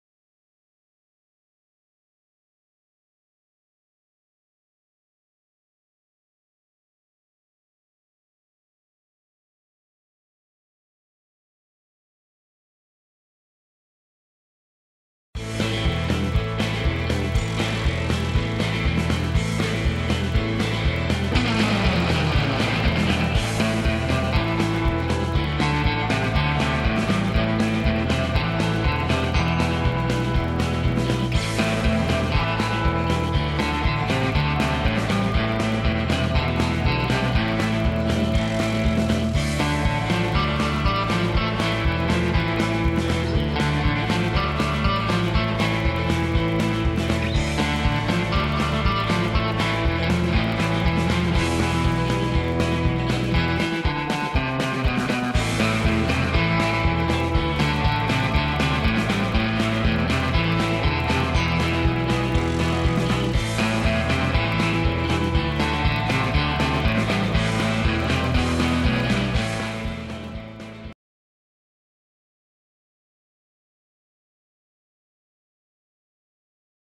Siku 6762 John Deere Special Trailer 8430T radio controlled
Siku 6762 John Deere 8430T tracked tractor radio controlled. This is real high tech!